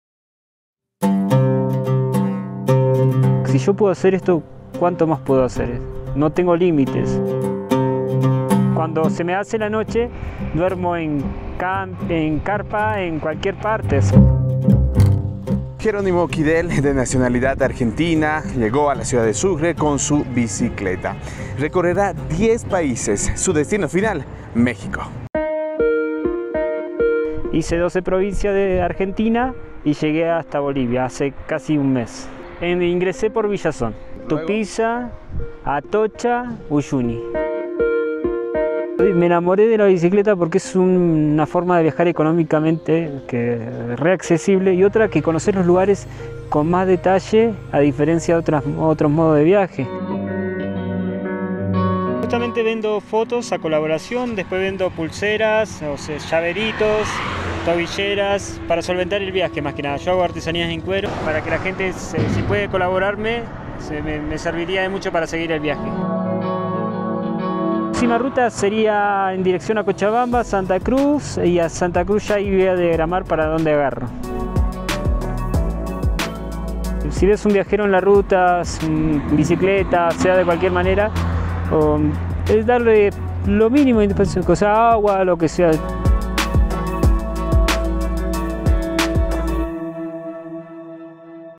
Rumbo a México en bicicleta
Gerónimo Quidel es un turista que viaja desde la Patagonia Argentina hasta México montado en una bicicleta, llegó a Sucre y nos cuenta su travesía por cumplir un sueño.